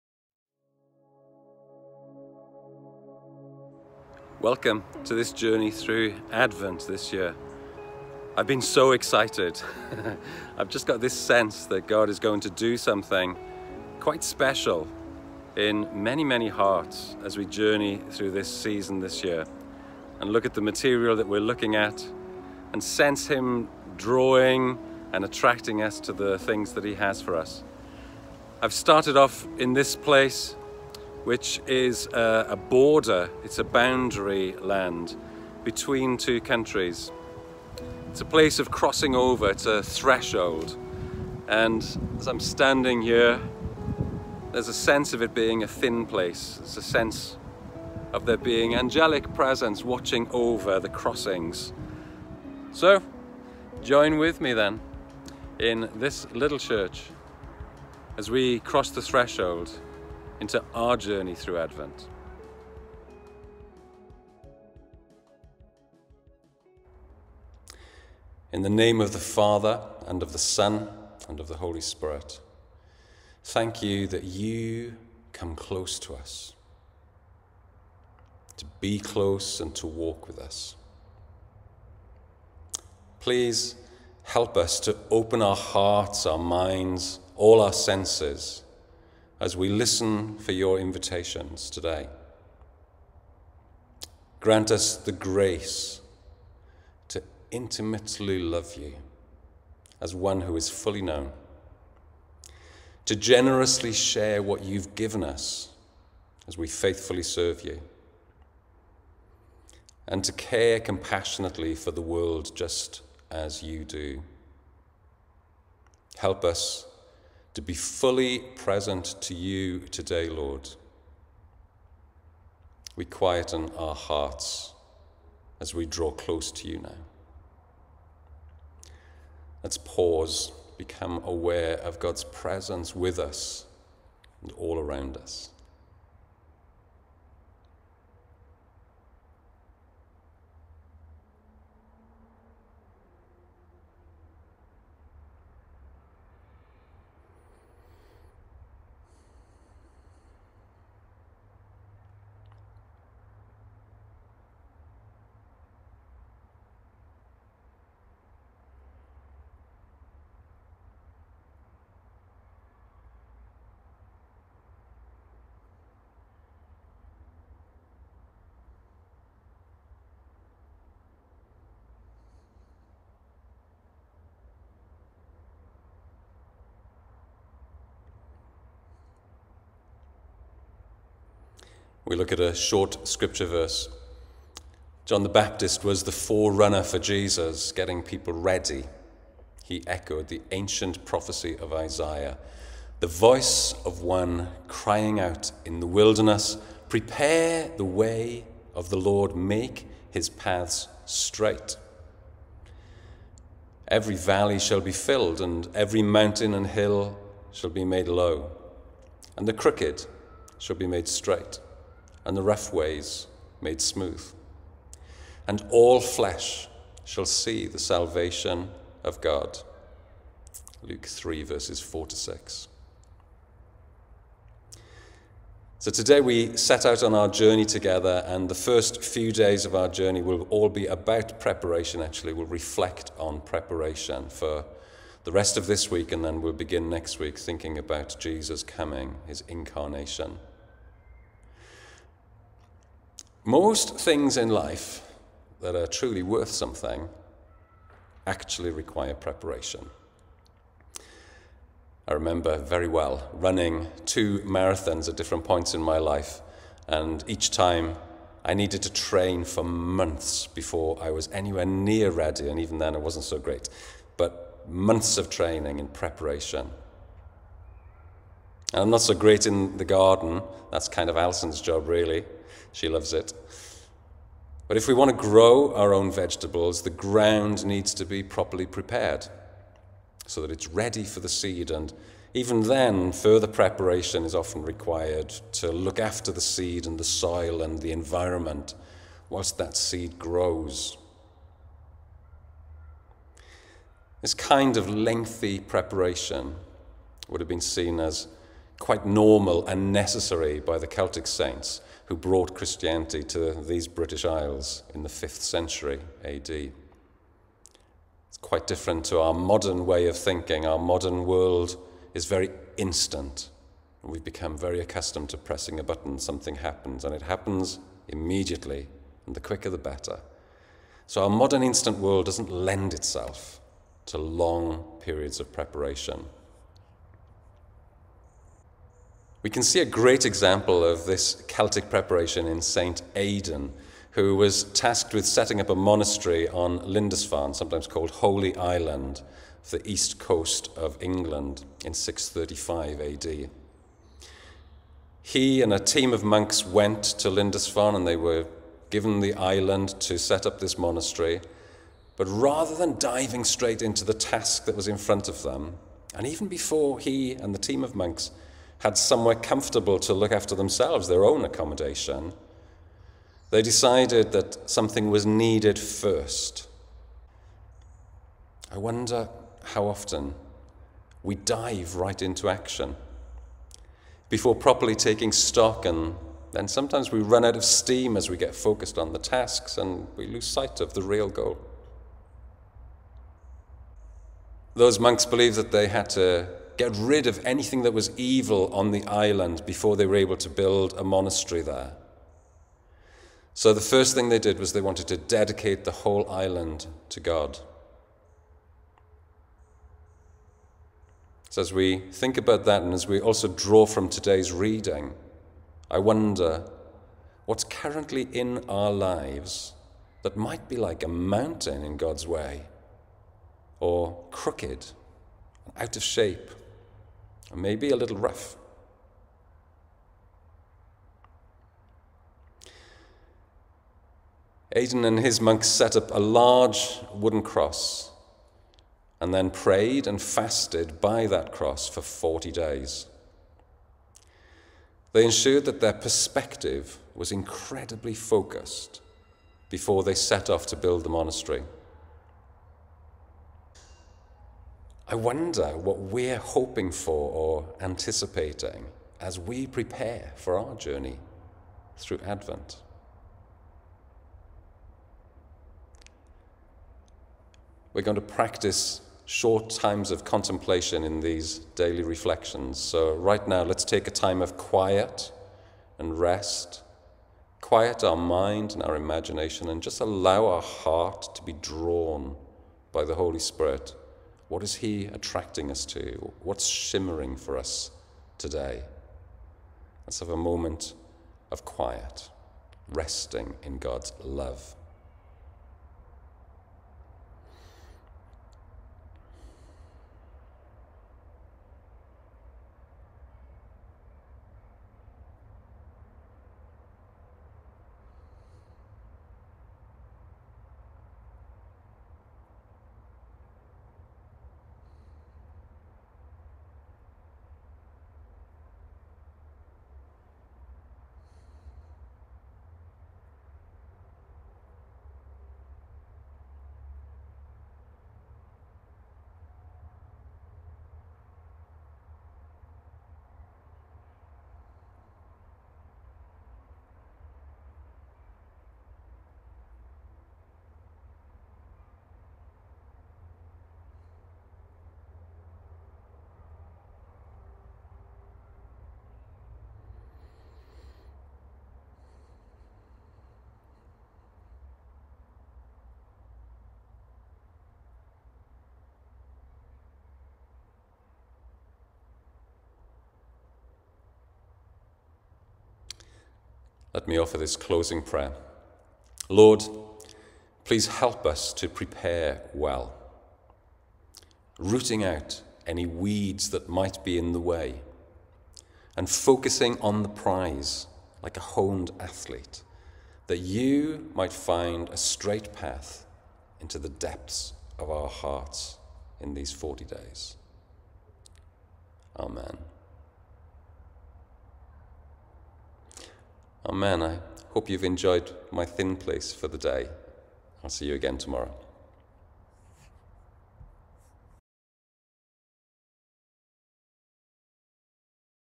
40 Days of Advent - 15th Nov
Wednesday November 15th
Preparation

Opening Prayer
Thank you that You come to us - to be close and to walk with us.
Please help me to open my heart, my mind and all my senses
as I listen for your invitations today
"Grant me the grace:
  to intimately love you, as one who is fully known;
  to generously share what you've given me, as I faithfully serve You;
  and to care compassionately for the world around me, just as You do."
Help me to be fully present to You today Lord
- I quieten my heart as I draw close to You now

Pause – and become aware of God’s presence – in and all around us

Scripture
John the Baptist was the forerunner for Jesus, getting people ready. He echoed the ancient prophecy of Isaiah:

‘The voice of one crying out in the wilderness:
“Prepare the way of the Lord,
    make his paths straight.
Every valley shall be filled,
    and every mountain and hill shall be made low,
    and the crooked shall be made straight,
    and the rough ways made smooth;
and all flesh shall see the salvation of God.”’
Luke 3:4-6

Thoughts
Most things in life that are truly worth something, will require preparation.
If we want to run a marathon, then months of training will likely be needed.
If we want to grow our own vegetables, the ground needs to be properly prepared so that it’s ready for the seed … and often further preparation is needed to tend the soil and the environment during the waiting and growing phase too.

This kind of lengthy preparation would have been seen as normal and necessary by the Celtic Saints who brought Christianity to the British Isles in the 5th century. Our modern ‘instant’ world doesn’t always help us in this regard!

We can see a great example of this ‘Celtic’ preparation in St Aidan, who was tasked with setting up a monastery on Lindisfarne in 635AD. Rather than diving straight into the task in front of him, and even before he and his team of monks had focused on making themselves comfortable with their own accommodation, Aidan decided that there was something special that had to be done first. I wonder how often we dive right into action before properly taking stock and then run out of steam as we get focused on the task(s), and lose sight of the real goal?

They believed that they had to cast out any evil that might live on the island, before building a monastery there – and they’d do that by first dedicating the island to God. Drawing also from today’s reading, I wonder what’s currently in our own lives that might be like a mountain in God’s way, or crooked and out-of-shape, or maybe a little rough?

Aidan and his monks set up a large wooden cross, then prayed and fasted by it for 40 days. They ensured that their perspective was incredibly focused before they set off to build the monastery. I wonder what we’re hoping for or anticipating as we prepare for our journey through Advent?

Contemplation
In a time of quiet and rest now, let’s allow the Holy Spirit to highlight to our hearts anything that is an invitation to us.

Allow at least a minute of silence for contemplation.

Closing Prayer
Lord, please help us to prepare well,
Rooting out any weeds that might be in the way,
And focusing on the prize like a honed athlete
- that you might find a ‘straight path’
into the depth of our hearts in these 40 days.
Amen